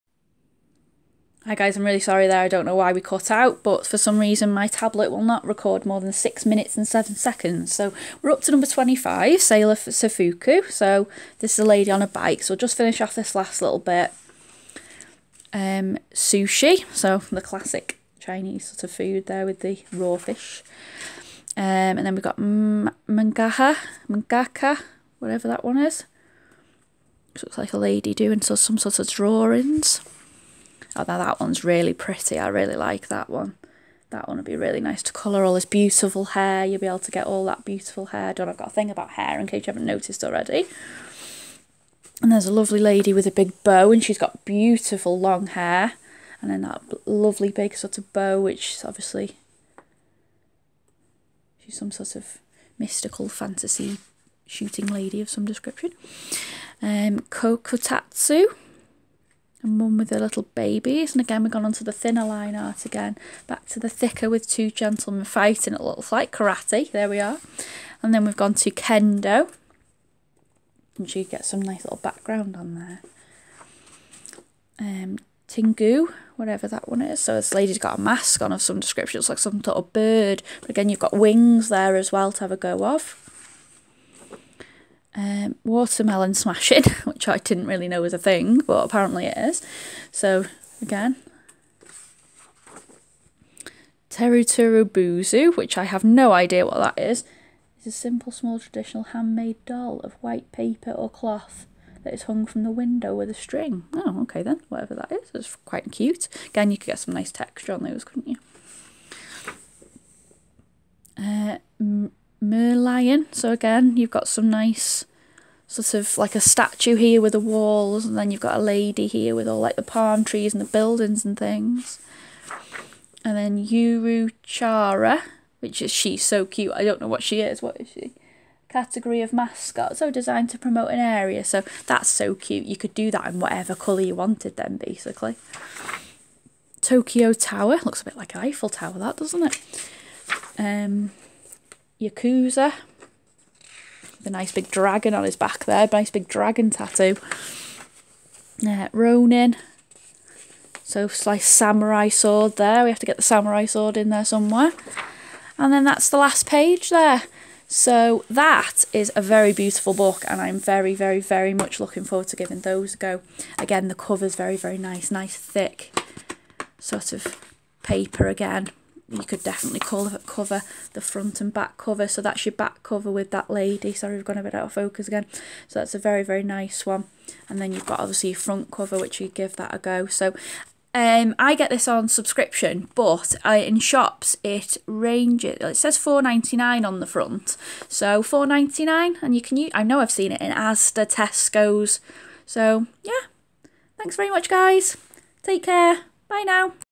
World of anime colouring heaven part 2
Part two of my colouring heaven review. Sorry got split up for some reason haha cx